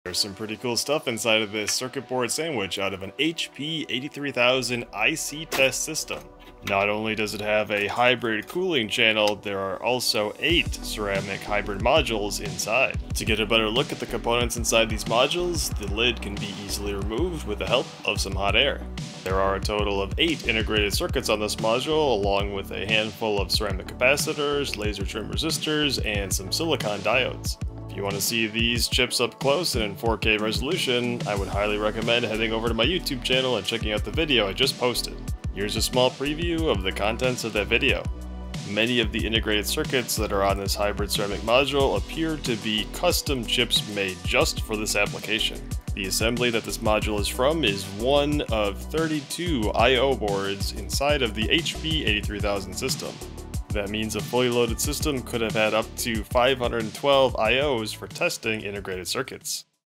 CCU #300: HP83000 I/O Hybrid Ceramic Module
I recently took a look at the Hybrid ceramic modules from inside an HP83000 IC test system. There are lots of interesting integrated circuits on these modules. If you want to see more of them under the microscope and at 4K resolution, check out the full video over on my channel.